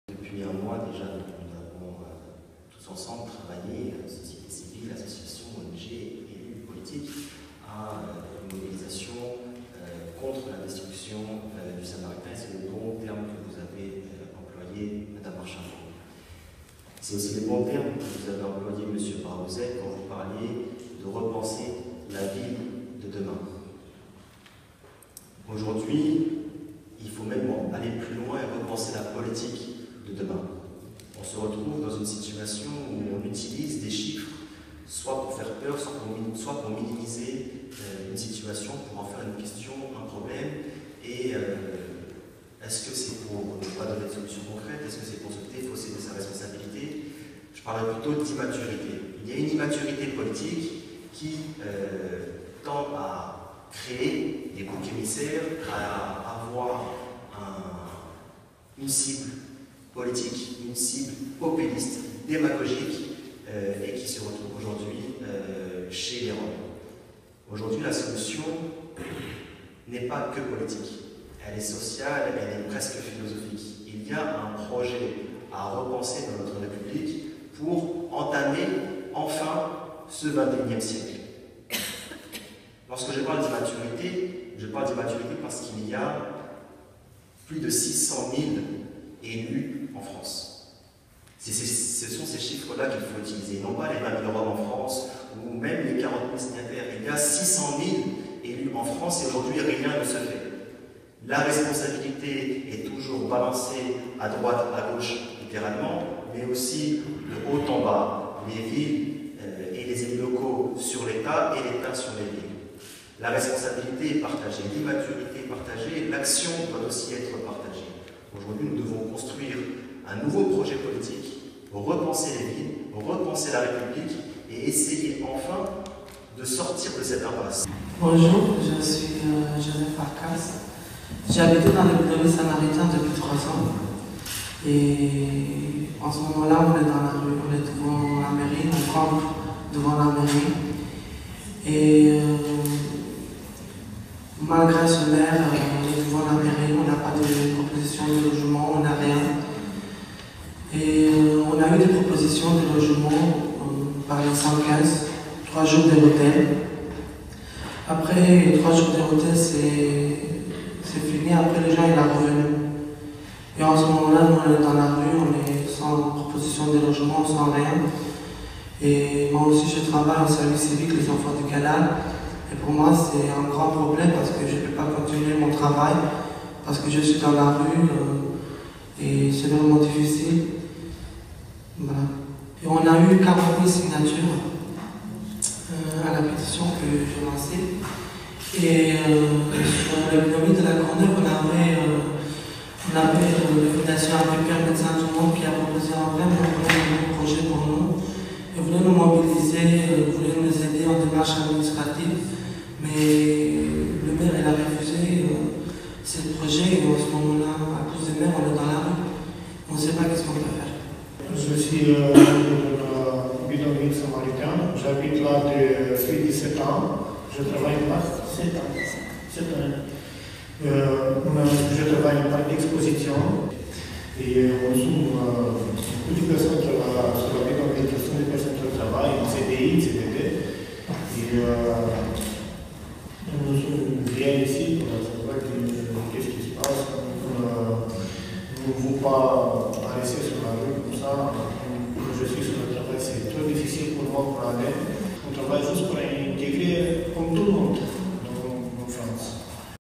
Conference de Presse au Sénat : Mehdi Bouteghmes & Jozsef Farkas
Extraits de la conférence de presse au Sénat organisée par la sénatrice Aline Archimbaud, le 10 septembre 2015. Mehdi Bouteghmes, conseiller municipal à la Courneuve : "il y a une immaturité politique, avec les Roms comme bouc émissaire", Jozsef Farkas : "on avait un projet et maintenant on est dans la rue". Iulian Minzat : "on travaille comme tout le monde pour s'intégrer en France".  De gauche à droite : Anina Ciuciu (Voix des Roms), Iulian Minzat (Samaritain), Patrick Braouezec (Plaine Commune), Aline Archimbaud (Senat), Jozsef Farkas (Samaritain), Medhi Bouteghmes (élu à La Courneuve), Grégoire Cousin (MigRom).